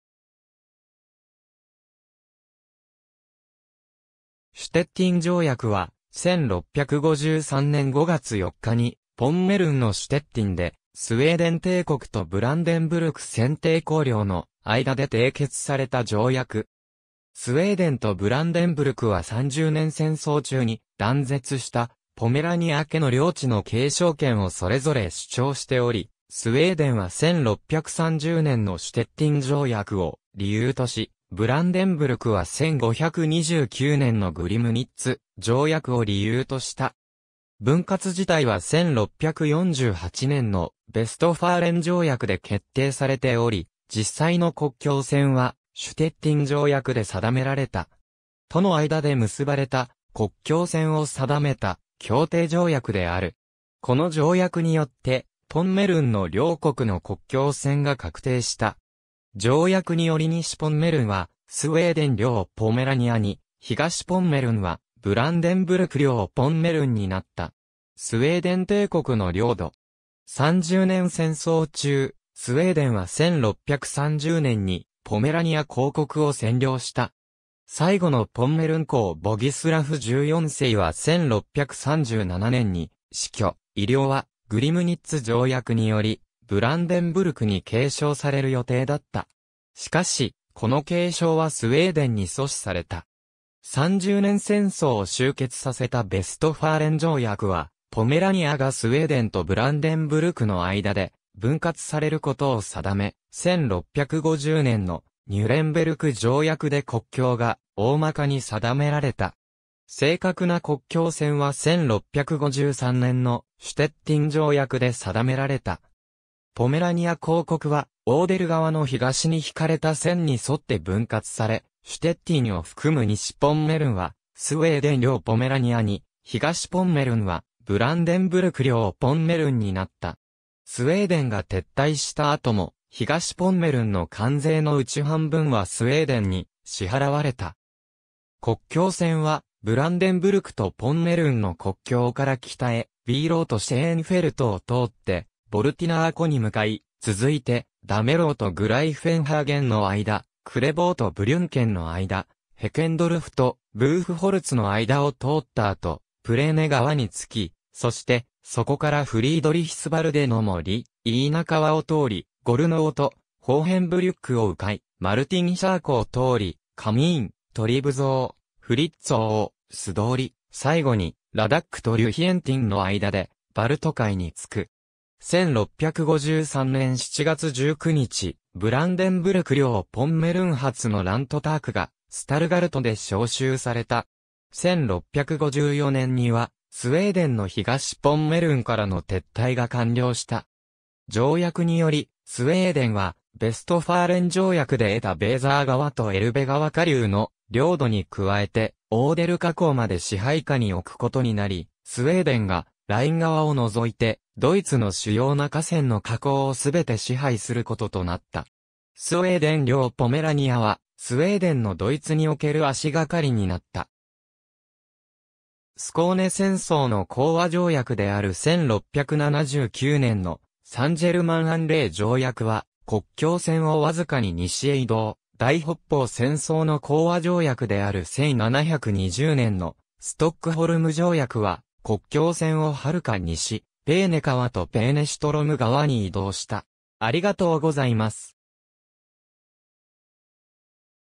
シュテッティン条約 (1653年)
ブランデンブルク＝プロイセンの条約
シュテッティン条約（シュテッティンじょうやく、ドイツ語: Grenzrezeß von Stettin）は、1653年5月4日にポンメルン（ポメラニア公国）のシュテッティンでスウェーデン帝国とブランデンブルク選帝侯領の間で締結された条約。
スウェーデンとブランデンブルクは三十年戦争中に断絶したポメラニア家の領地の継承権をそれぞれ主張しており、スウェーデンは1630年のシュテッティン条約を理由とし、ブランデンブルクは1529年のグリムニッツ条約を理由とした。
分割自体は1648年のヴェストファーレン条約で決定されており、実際の国境線はシュテッティン条約で定められた。
との間で結ばれた国境線を定めた協定条約である。
この条約によってポンメルンの両国の国境線が確定した。
条約により西ポンメルンはスウェーデン領ポメラニアに、東ポンメルンはブランデンブルク領ポンメルンになった。
スウェーデン帝国の領土。
三十年戦争中、スウェーデンは1630年にポメラニア公国を占領した。
最後のポンメルン公ボギスラフ14世は1637年に死去、遺領はグリムニッツ条約によりブランデンブルクに継承される予定だった。
しかし、この継承はスウェーデンに阻止された。
三十年戦争を終結させたヴェストファーレン条約はポメラニアがスウェーデンとブランデンブルクの間で分割されることを定め、1650年のニュレンベルク条約で国境が大まかに定められた。
正確な国境線は1653年のシュテッティン条約で定められた。
ポメラニア公国はオーデル川の東に引かれた線に沿って分割され、シュテッティンを含む西ポンメルンはスウェーデン領ポメラニアに、東ポンメルンはブランデンブルク領ポンメルンになった。
スウェーデンが撤退した後も、東ポンメルンの関税のうち半分はスウェーデンに支払われた。
国境線はブランデンブルクとポンメルンの国境から北へ（グライフェンハーゲンとヴィルデンブルッフはスウェーデン領に残る）、ヴィーローとシェーンフェルト（Schönfeld）を通ってヴォルティナー湖（Woltiner See）に向かい、続いてダメローとグライフェンハーゲンの間、クレボーとブリュンケン（Brünken）の間、ヘケンドルフとブーフホルツの間を通った後、プレーネ川に着き、そしてそこからフリードリヒスヴァルデの森、イーナ川を通り、ゴルノウとホーヘンブリュックを迂回（両方ともスウェーデン領）、マルティンシャー湖（Martinscher See）を通り、カミーン、トリブゾー、フリッツォーを素通り（いずれもスウェーデン領）、最後にラダック（Raddack）とリュヒェンティン（Lüchentin）の間でバルト海に着く。
1653年7月19日、ブランデンブルク領ポンメルン初のラントタークがスタルガルトで招集された。
1654年にはスウェーデンの東ポンメルンからの撤退が完了した。
条約により、スウェーデンはヴェストファーレン条約で得たヴェーザー川とエルベ川下流の領土に加えて、オーデル河口まで支配下に置くことになり、スウェーデンがライン川を除いてドイツの主要な河川の河口を全て支配することとなった。
スウェーデン領ポメラニアはスウェーデンのドイツにおける足掛かりになった。
スコーネ戦争の講和条約である1679年のサン＝ジェルマン＝アン＝レー条約は国境線を僅かに西へ移動、大北方戦争の講和条約である1720年のストックホルム条約は国境線を遥か西、ペーネ川とペーネシュトロム川に移動した。